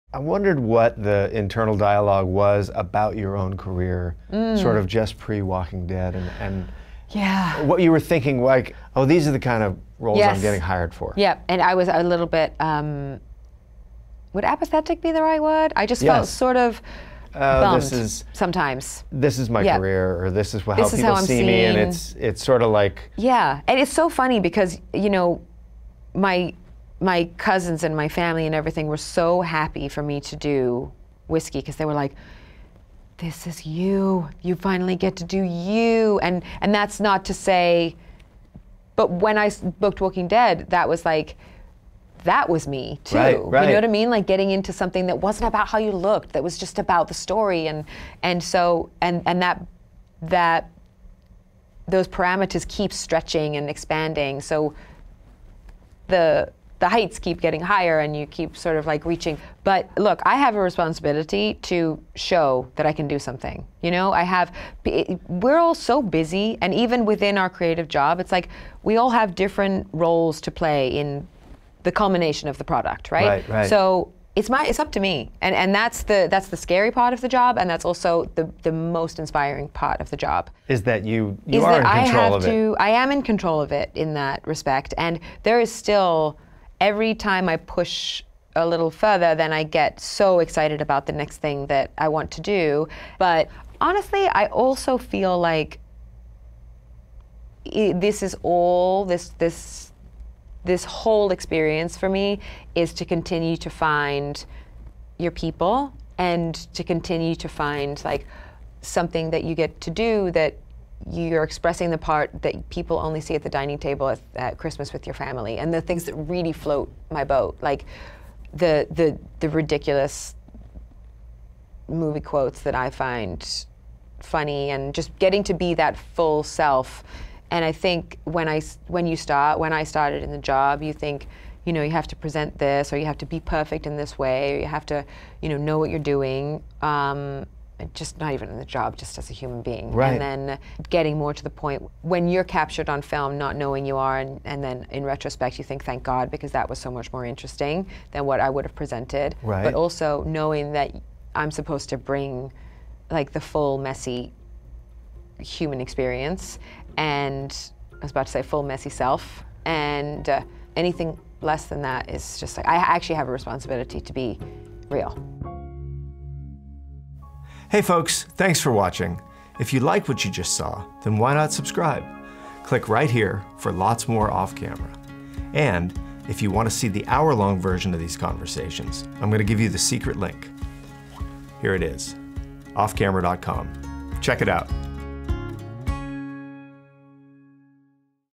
Lauren Cohan Feels the Responsibility to Be Real
Lauren Cohan from The Walking Dead and now Whiskey Cavalier finally feels like she's been given a few roles where she can truly express herself, but she understands that it's her responsibility to bring that forward.